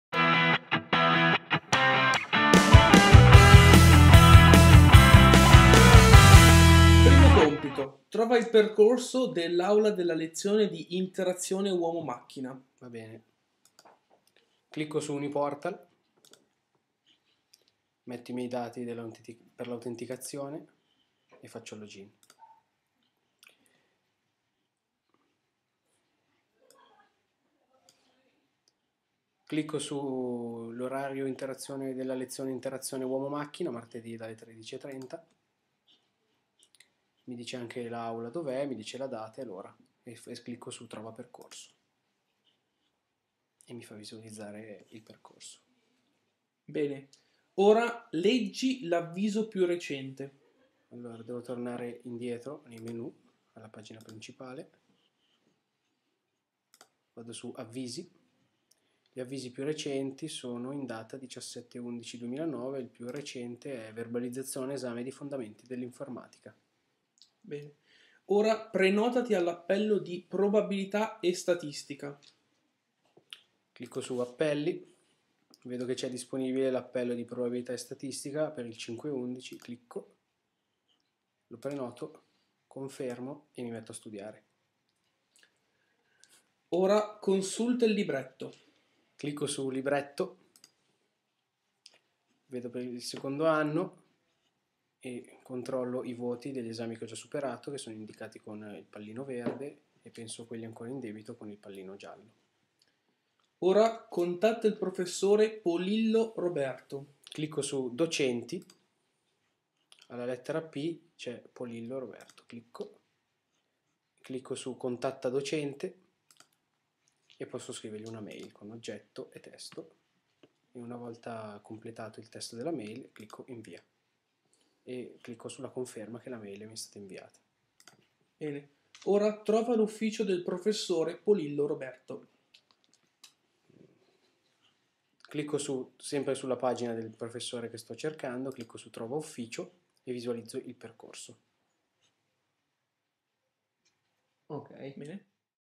Progetto di IUM: prototipo cliccabile 2 di 4
Progetto di Interazione Uomo-Macchina: prototipo cliccabile 2 di 4.
Alessandro testa il prototipo di carta dell'applicazione Uniportal: app per iPhone che permette di accedere velocemente alle risorse dell'università senza passare per il sito.

Progetto: UniPortal
Gruppo: 3gGroup
Componenti: Giuseppe Pogliani, Giuseppe Magno, Giuseppe Maggi

Università Milano-Bicocca
Corso: Interazione Uomo Macchina
Docente: Polillo Roberto